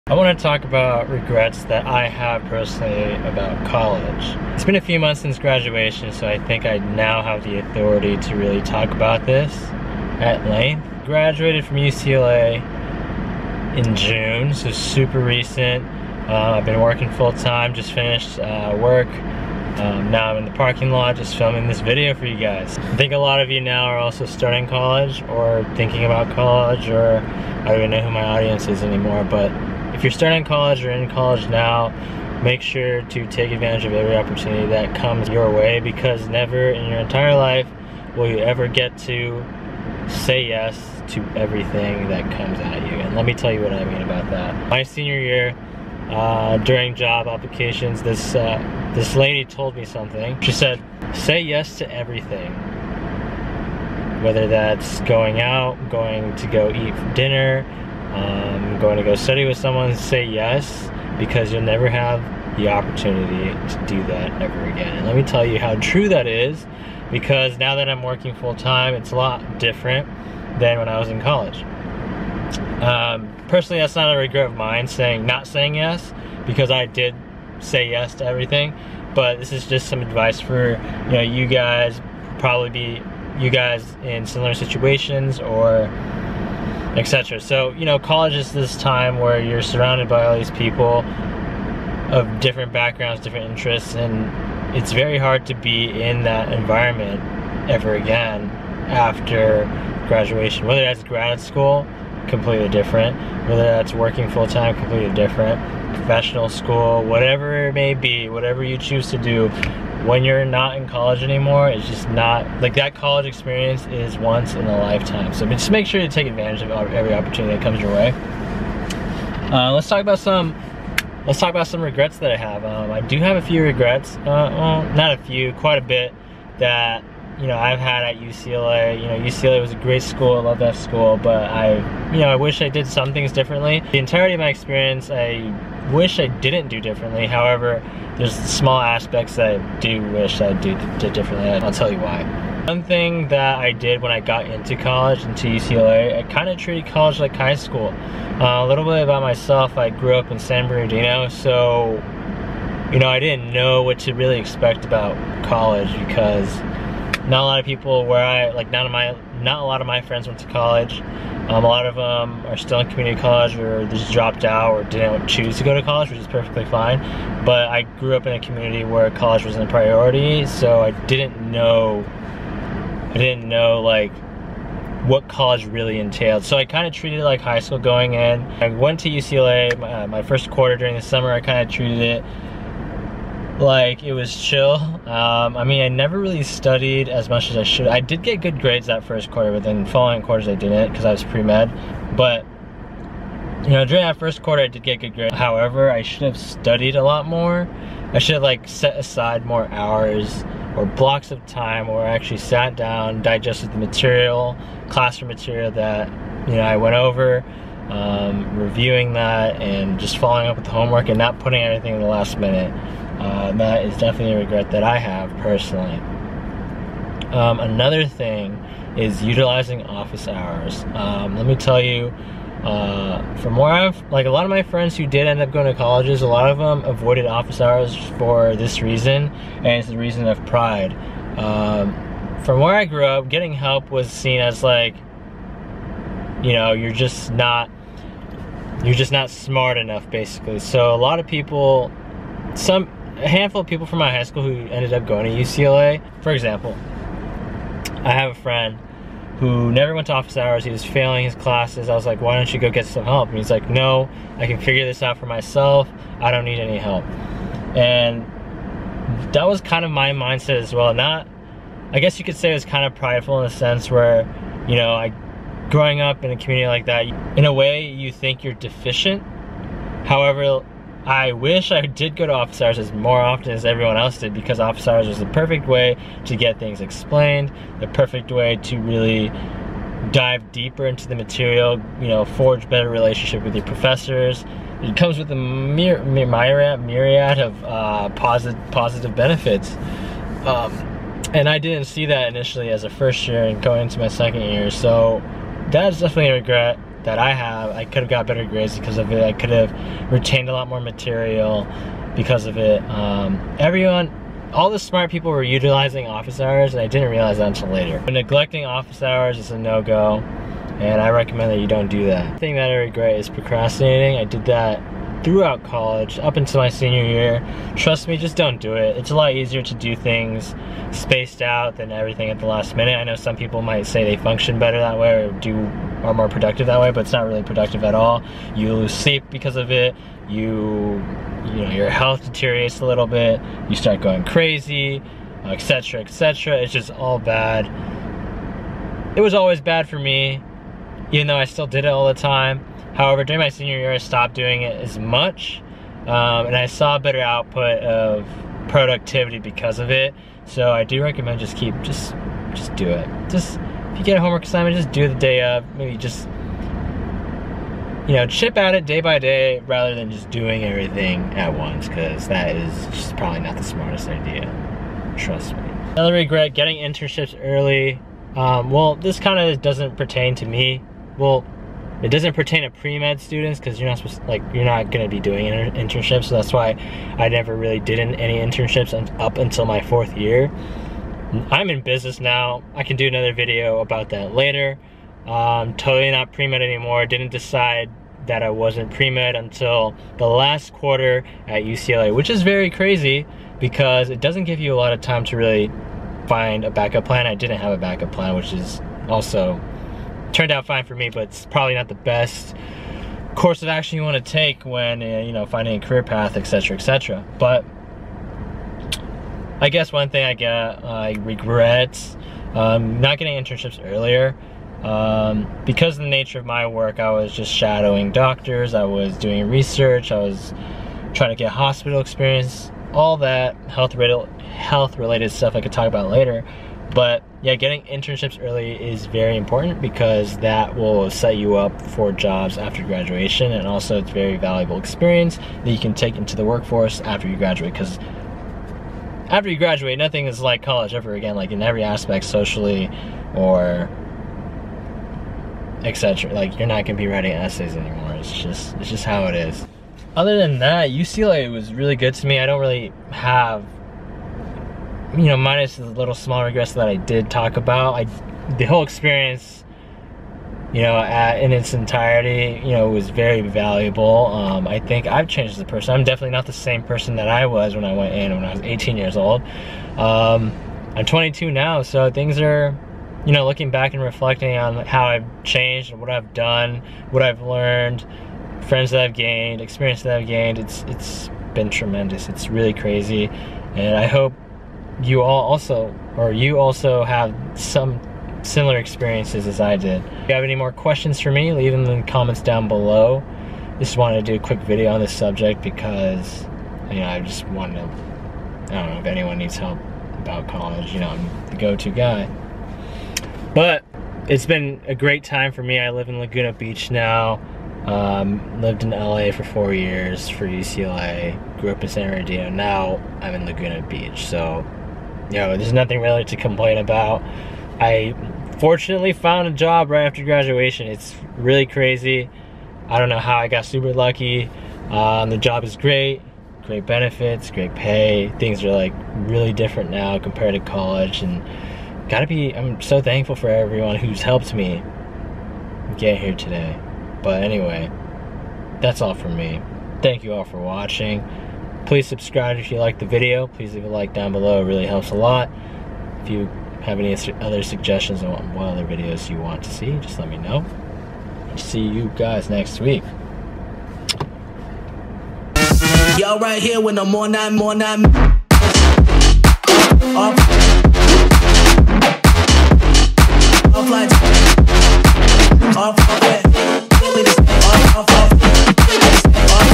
My Regrets As a Pre-Med Student at UCLA
Back when I was a pre-med student at UCLA, there are a few things I could have done differently (and a few things I wish I knew) to learn more, to retain more information, and to improve my skills on my path to medicine.

MY GEAR
Blue Yeti microphone

*Filmed with a Canon G7X